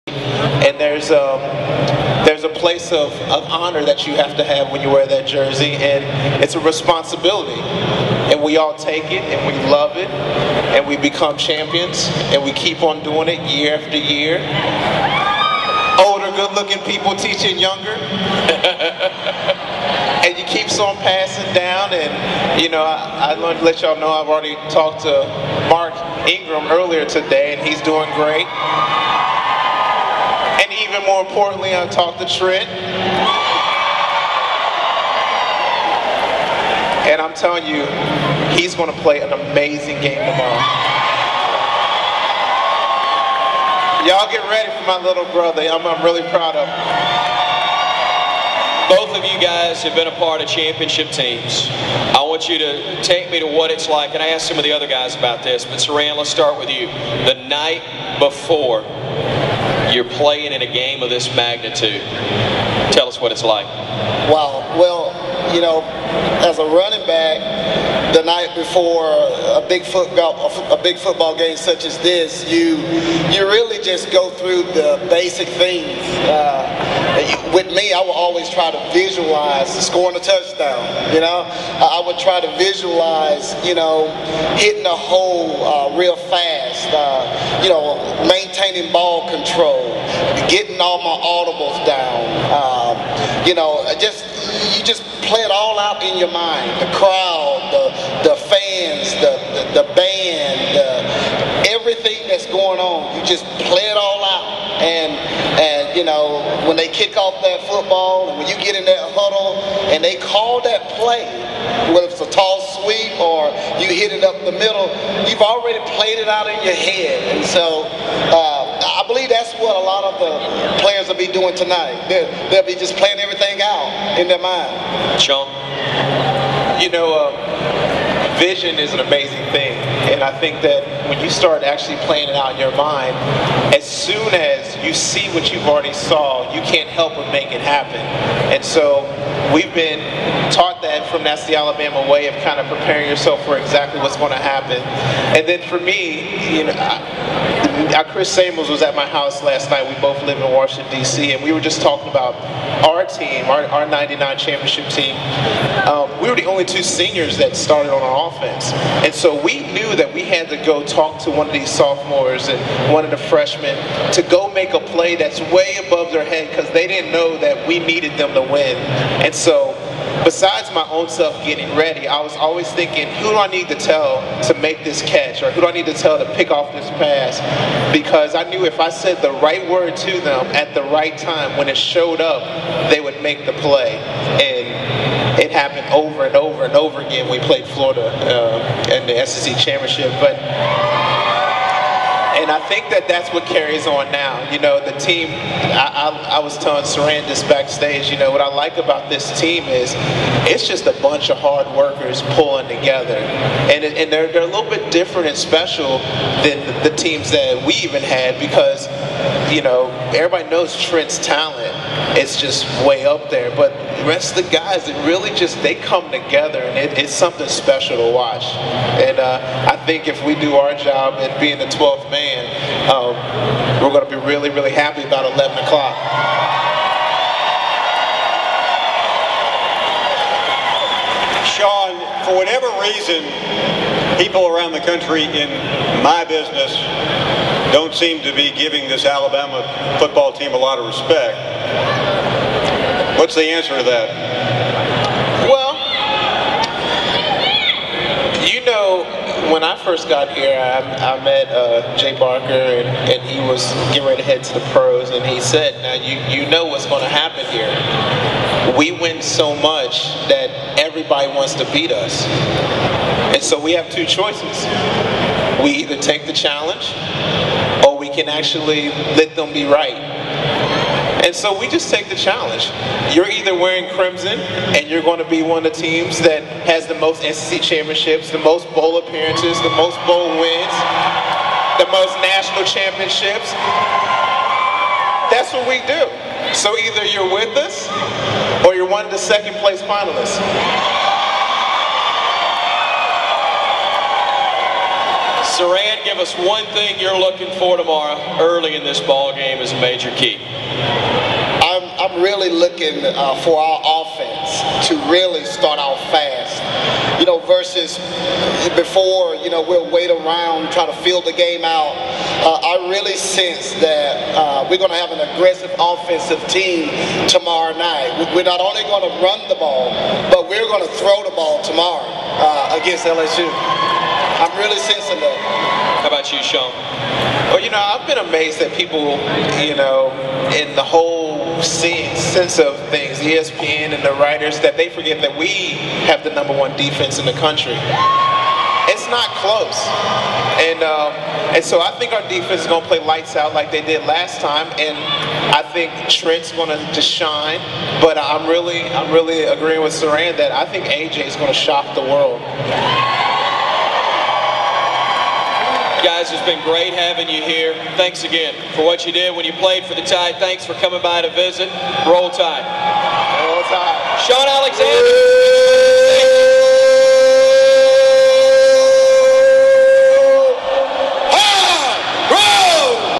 Shuan Alexander and Siran Stacy -- Bama Bowl Bash New Orleans
Love it!! Roll Tide.

Two great Alabama football stars enlighten.